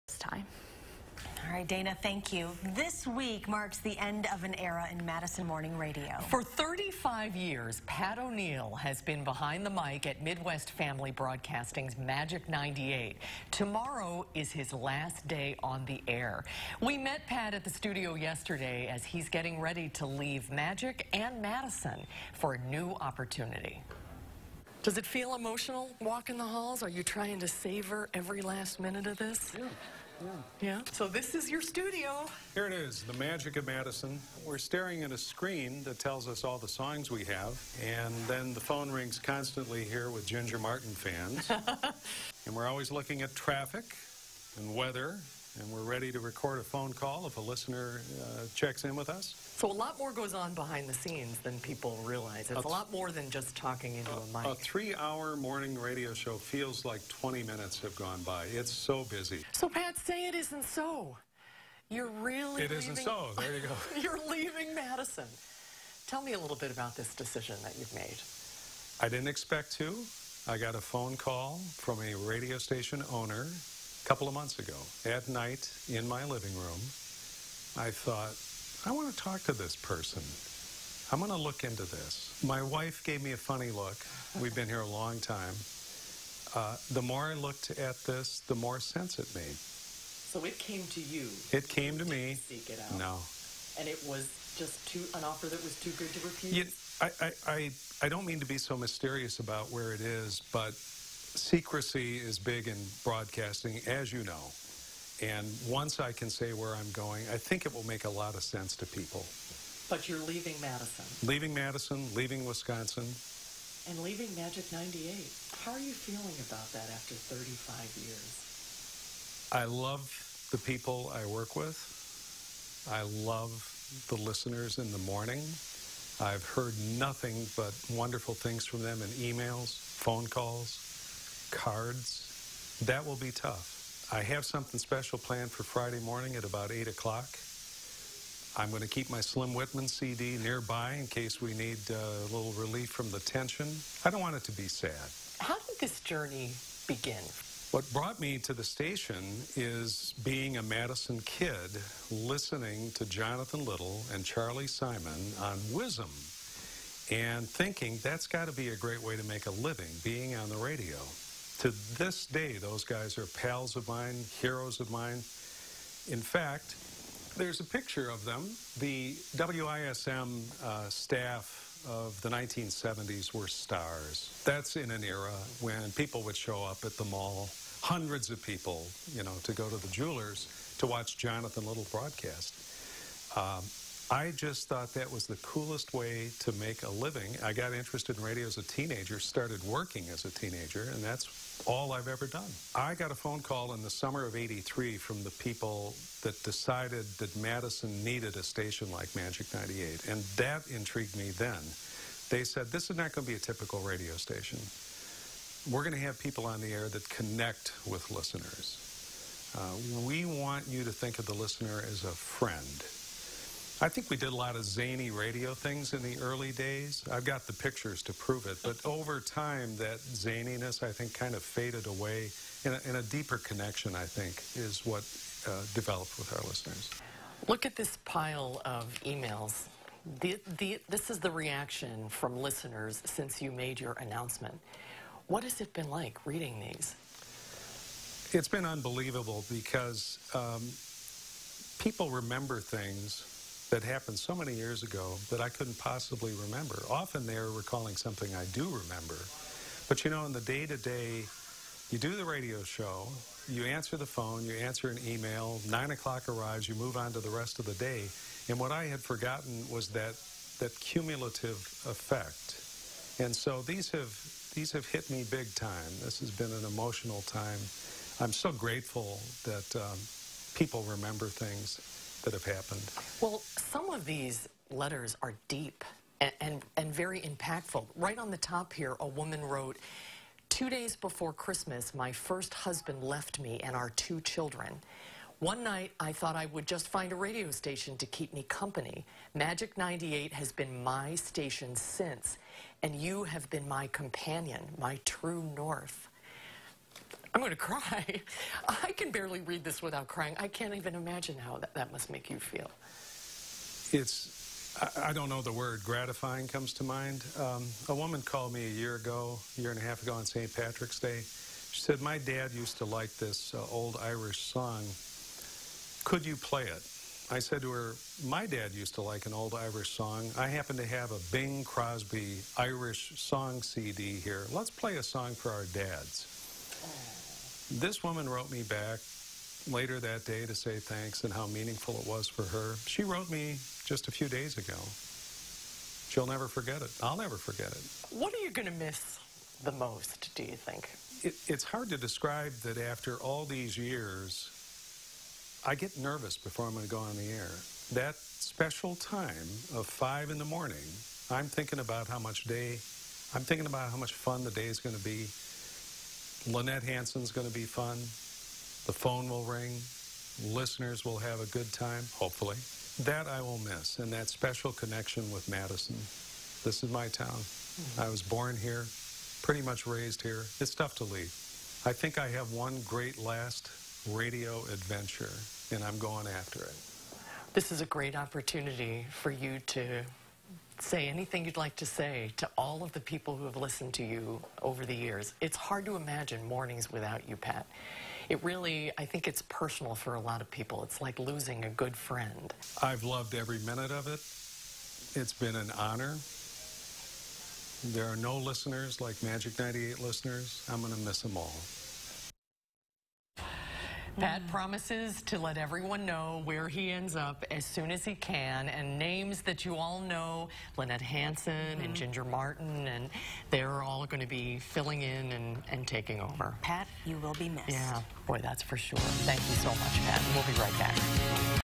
Magic 98's Pat O'Neill moving on after 35 years
Magic 98's Pat O'Neill is moving on after 35 years behind the mic.